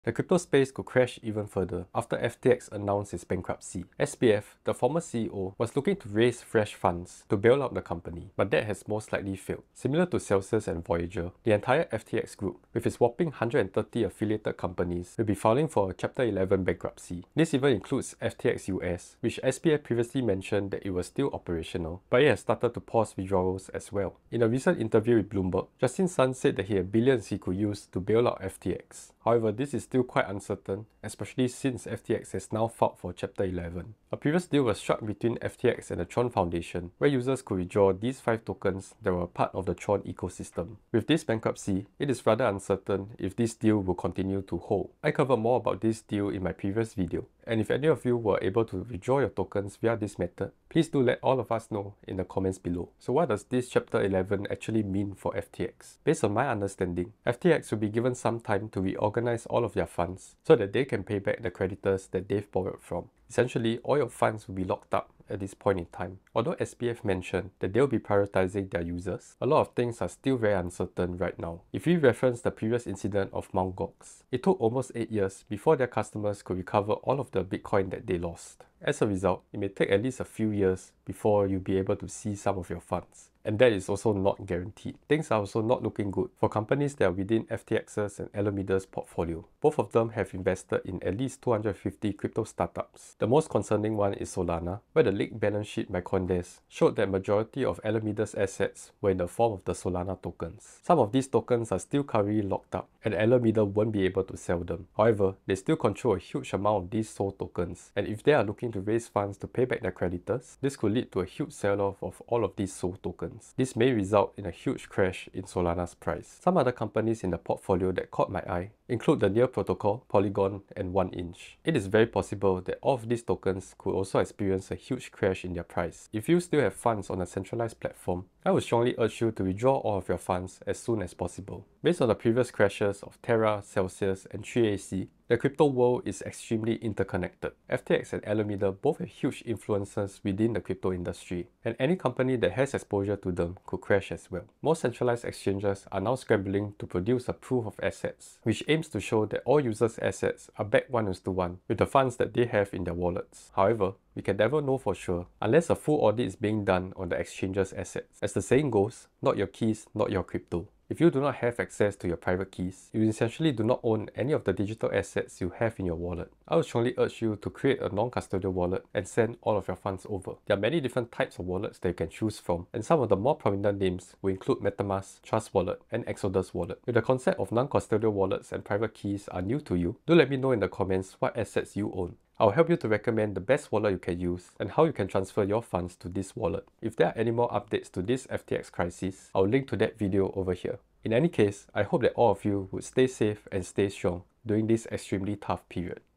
What's NEXT After FTX’s Bankruptcy? (Do THIS Now)
After filing for a Chapter 11 bankruptcy, things aren't looking good for FTX and Alameda.

As a precaution, I would strongly suggest that you withdraw all funds from any centralised platform (exchange or lending).